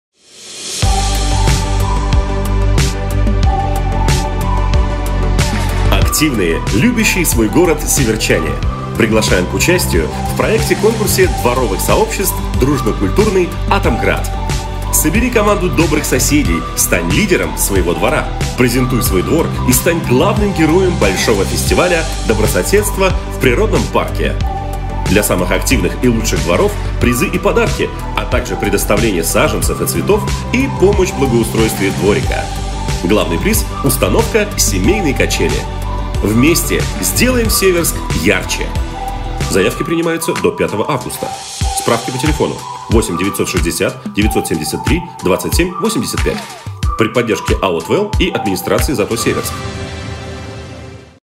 Дружно-культурный Атомград
Общественная организация содействия развитию молодёжи и семьи «Верный путь» при поддержке АО ТВЭЛ, Администрации ЗАТО Северск представляет городской проект-конкурс дворовых сообществ дружно-культурный АТОМГРАД.
Целью проекта является поддержка и развитие местных соседских сообществ на территории Северска. Местные соседские сообщества - жители дома или группы домов микрорайона.
Приглашаем к участию активных лидеров сообществ и инициативные группы-представители от дворов. Соберите команду добрых соседей, подготовьте презентацию своего двора и организуйте День открытых дверей. Для организации Дня открытых дверей двора привлекается организационно – технический ресурс муниципальных учреждений, творческие коллективы города.
Подведение итогов состоится на фестивале добрососедства в Северском природном парке. Для самых активных и лучших дворов призы и подарки, предоставление саженцев и цветов, помощь в благоустройстве дворика. Лучшее соседское сообщество получит возможность финансирования на установку семейной качели.
Вместе сделаем свой двор и Северск красивее и дружнее!
Заявки принимаются до 5 августа. Все подробности и форма заявки в Положении. Количество заявок ограничено.